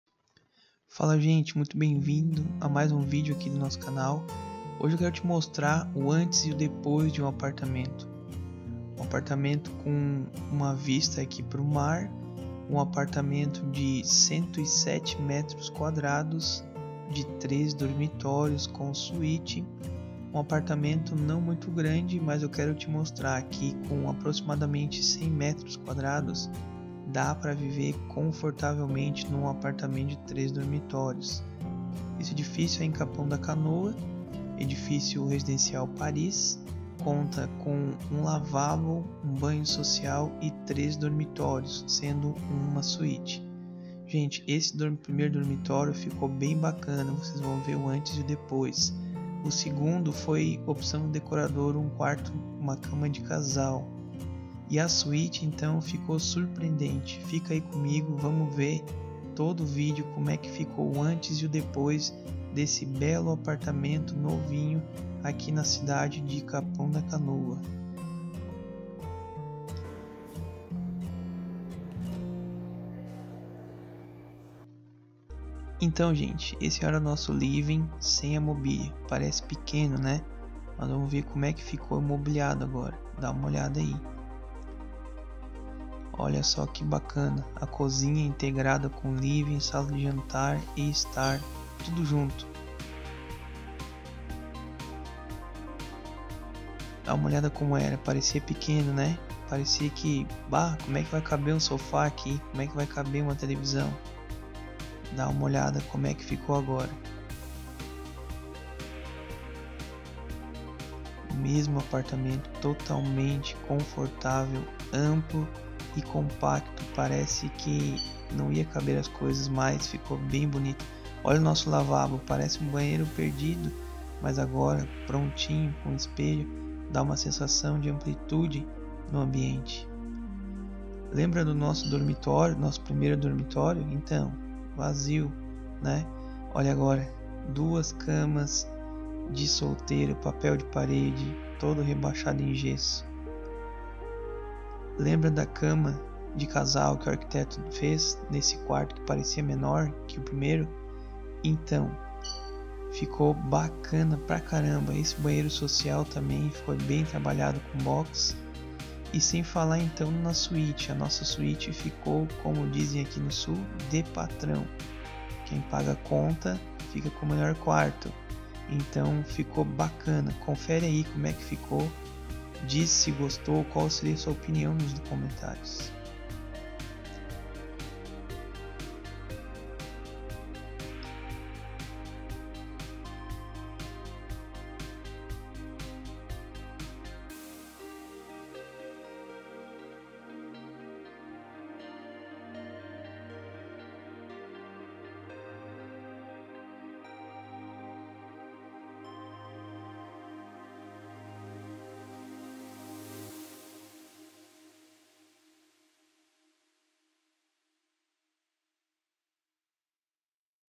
😍 DECORANDO UM AP DE 100M² ANTES E DEPOIS - SURPREENDA-SE!!!
ANTES E DEPOIS DE BELO APTO DE 3 DORMITÓRIOS COM 107M²

ED. PARIS - LOCALIZADO EM CAPÃO D ACANOA,

APARTAMENTO ENCONTRA-SE A VENDA!

R$ 983.000,00 (ATÉ 60 X PARA PAGAR)
ESTUDA RECEBER IMÓVEL DE MENOR VALOR

MAIORES INFORMAÇÕES: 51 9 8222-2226 WHATS